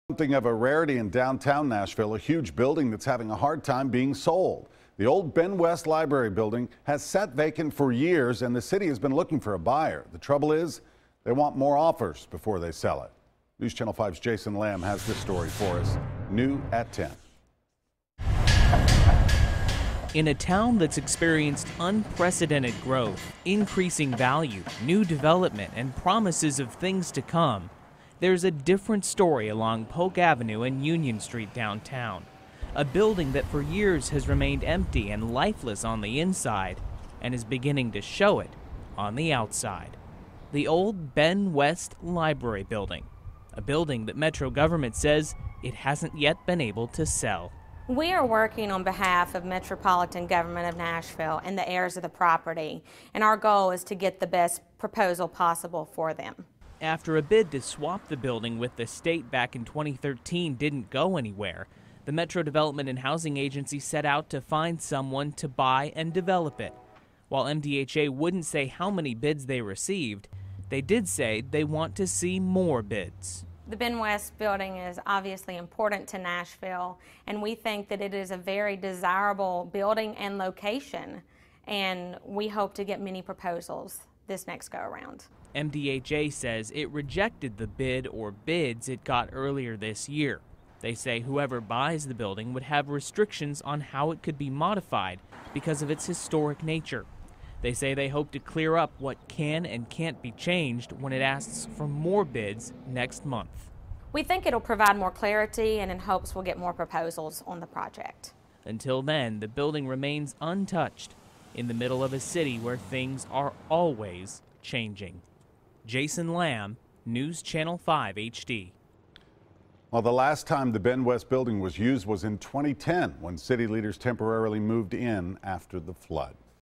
Downtown Nashville's Ben West Building Seeks Buyer
It's something of a rarity in downtown Nashville -- a huge building having a hard time being sold.